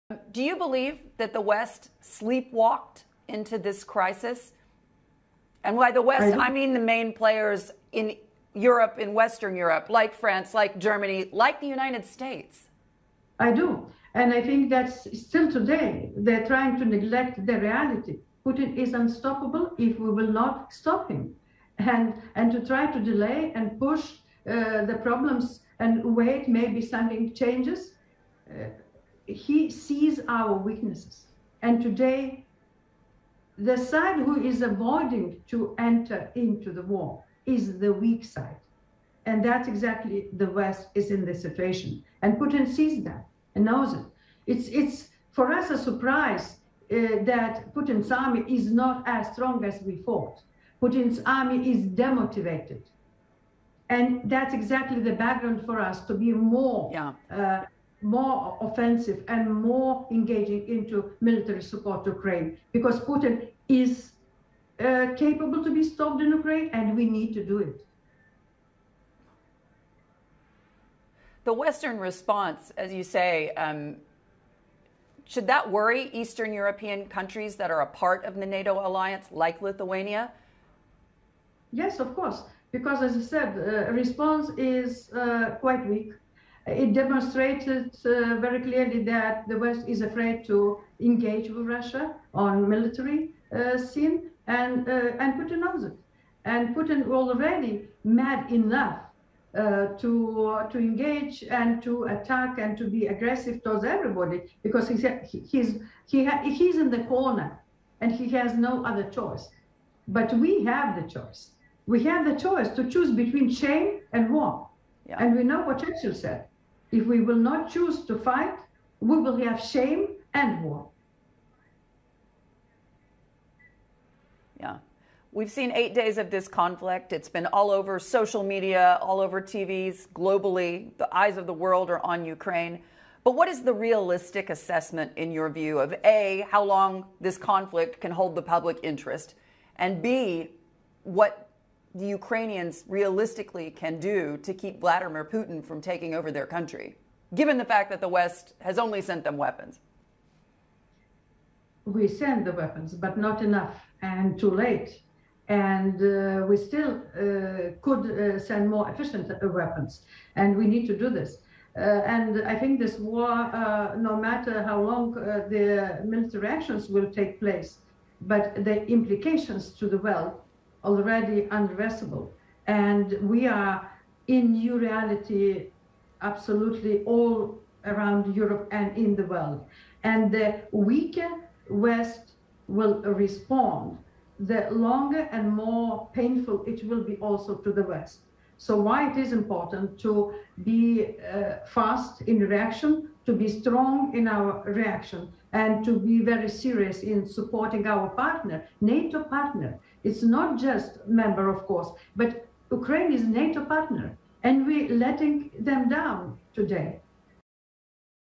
The West response to Russia is 'weak': Former President of Lithuania
"If we choose not to fight, we will have shame and war", says former President of Lithuania Dalia Grybauskaite who urges the West to help defend Ukraine militarily and include the country in European Union.